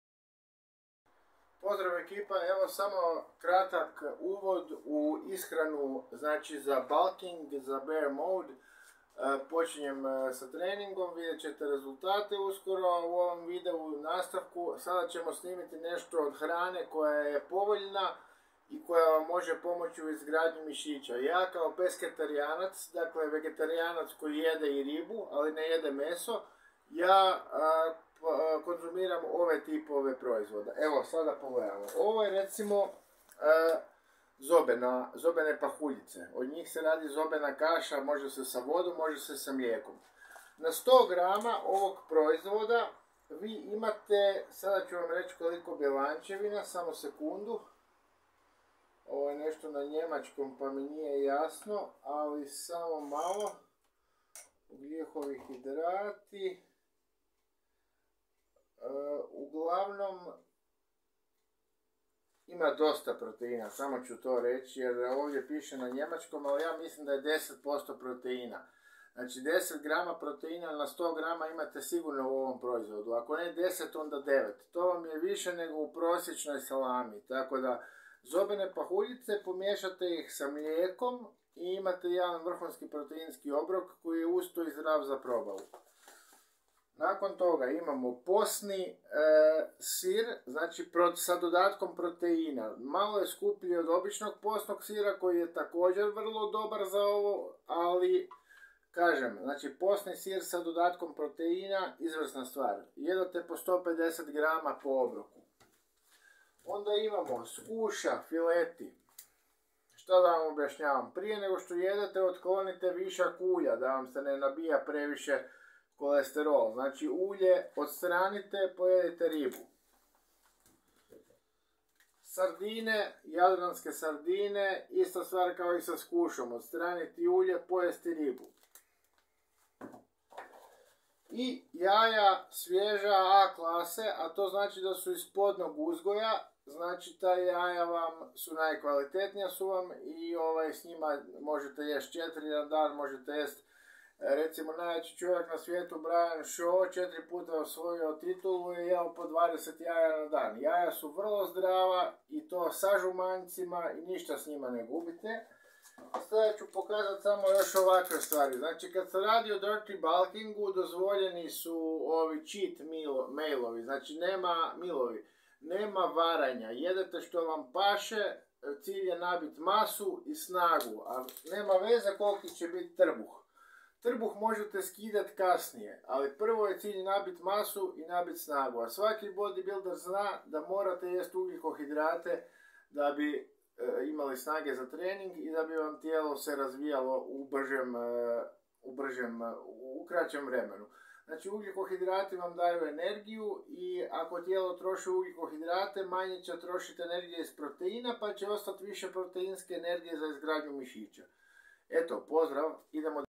Snaga i masa (proteinska prehrana - dirty bulk!)
Neke od dostupnih i jeftinih namirnica koje vam mogu biti od koristi, bilo da trenirate za snagu, za masu, kao bodybuilder, strongman, Street workout vjezbač, ili kao bilo koji sportaš, ove namirnice vam mogu itekako biti od koristi!